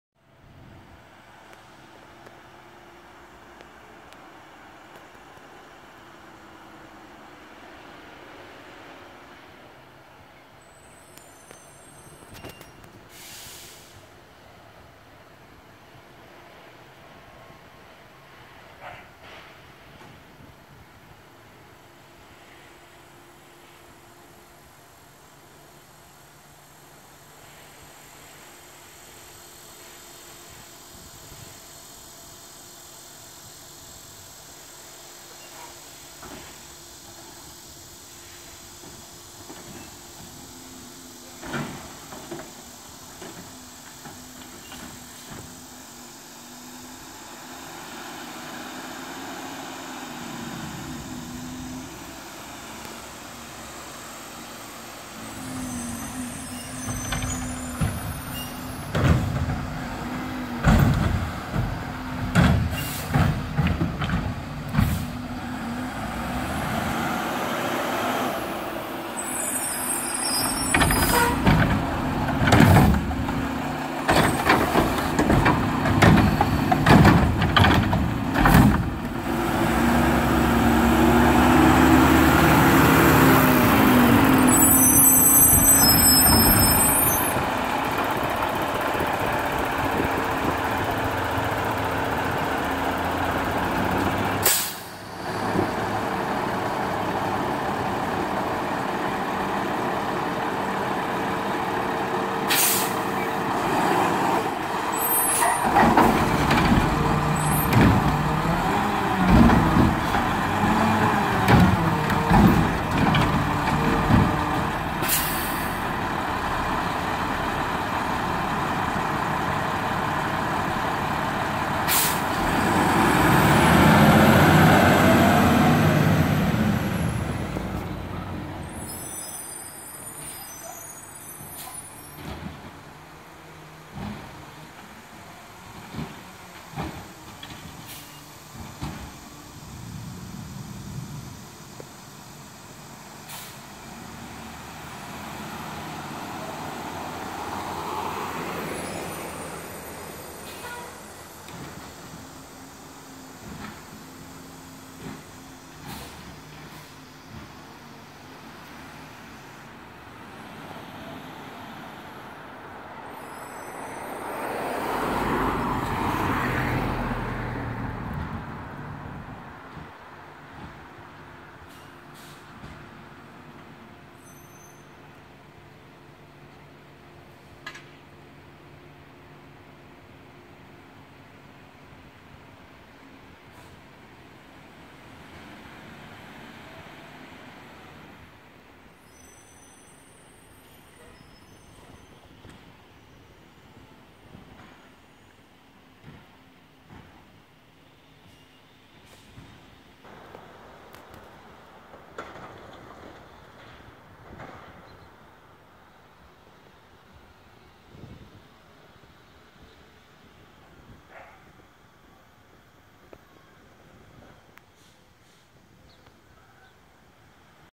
Republic service summer footage 2326 2019
In this video we see Ralph taking some trash. Believe it or not this truck is already one year old almost when this was filmed. Enjoy!